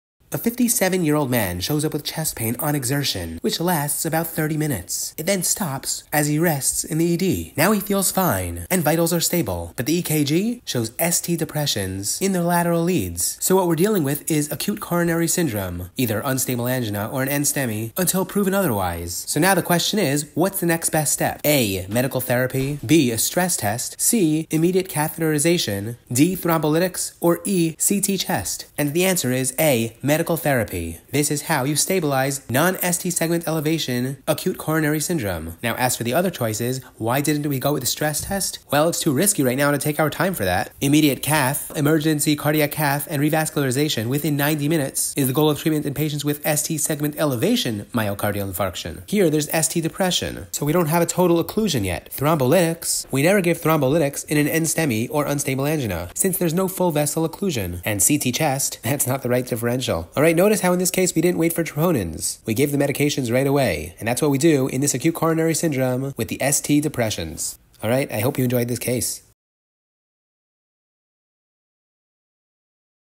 NSTEMI Management: Don’t Get This Wrong
A 57-year-old man walks in with chest pain and ST depressions. Vitals? Stable. Troponins? Pending. But don’t be fooled — this could be NSTEMI, and it needs immediate action.

In this fast-paced breakdown, we walk you through:
✅ How to recognize NSTEMI
✅ Why it’s dangerous even without dramatic ECG changes
✅ The right time for cath
✅ When NOT to give thrombolytics
✅ Key differences between NSTEMI and STEMI